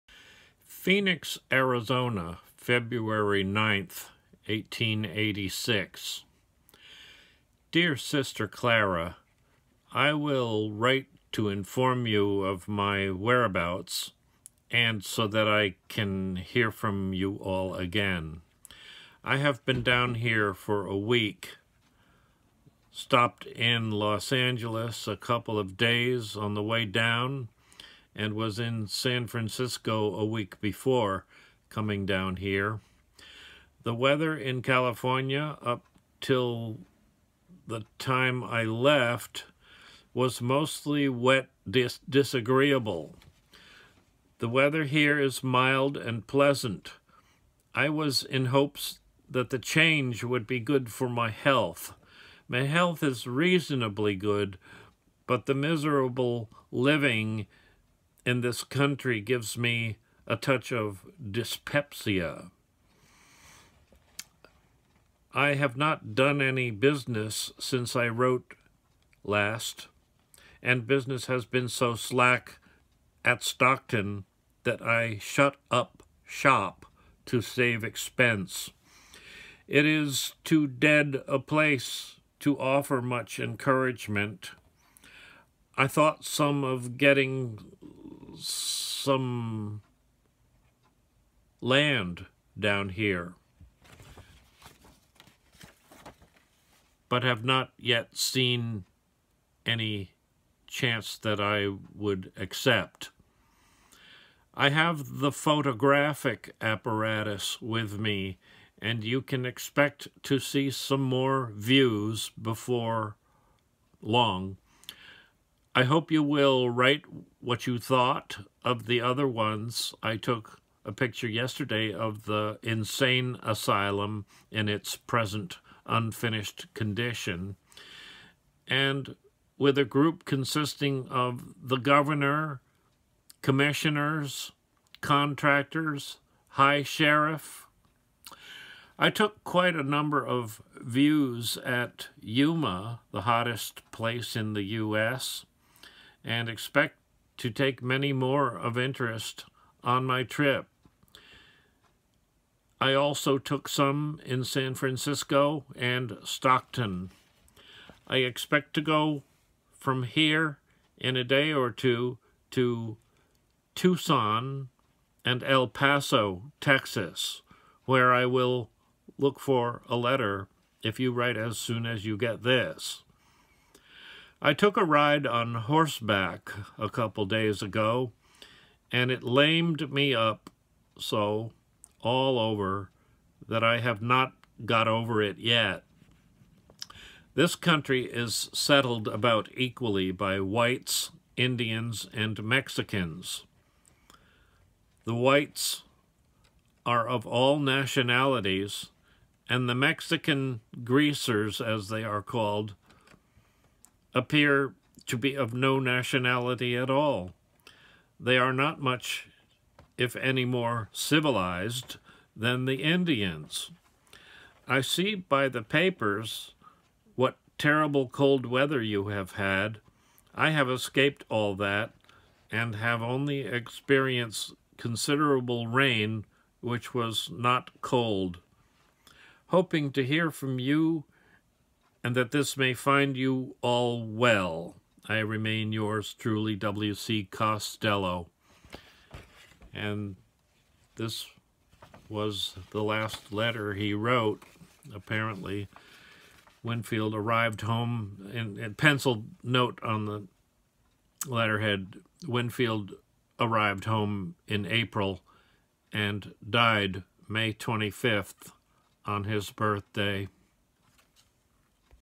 Winfield Costelow February 9, 1886 letter.
Phoenix, Arizona, February 9, 1886

Dear sister Clara,

I will write to inform you of my whereabouts and so that I can hear from you all again. I have been down here for a week. Stopped in Los Angeles a couple of days on the way down and was in San Francisco a week before coming down here. The weather in Cal. Up till the time I left was mostly wet disagreeable. The weather here is mild and pleasant. I was in hopes that the change would be good for my health. My health is reasonably good. But the miserable living in the country gives me a touch of dyspepsia. I have not done any business since I wrote last and business has been so slack at Stockton that I shut up shop to save expense. It is too dead a place to offer much encouragement. I thought some of getting some land down here but have not yet seen any chance that I would accept.
 I have the photographic apparatus with me and you can expect to see some more views before long. I hope you will write what you thought of the other ones. I took a picture yesterday of the insane asylum in its present unfinished condition and with a group consisting of the governor, commissioners, contractors, high sherriff. I took quite a number of views at Yuma, the hottest place in the U. S. and expect to take many more of interest on my trip. I also took some in San Francisco and Stockton. I expect to go from here in a day or two to Tucson and El Paso, Texas where I will look for a letter if you write as soon as you get this.
 I took a ride on horseback a couple days ago and it lamed me up so all over that I have not got over it yet.
 This country is settled about equally by whites, Indians and Mexicans. The whites are of all nationalities and the Mexicans or greasers as they are called appear to be of no nationality at all. They are not much if any more civilized than the Indians.
 I see by the papers what terriblr cold weather you have had. I have escaped all that and have only experienced considerable rain which was not cold. Hoping to hear from you and that this may find you all well.

 I remain yours truly,
 W. C. Costelow
Winfield arrived home in April and died May 25 on his birthday.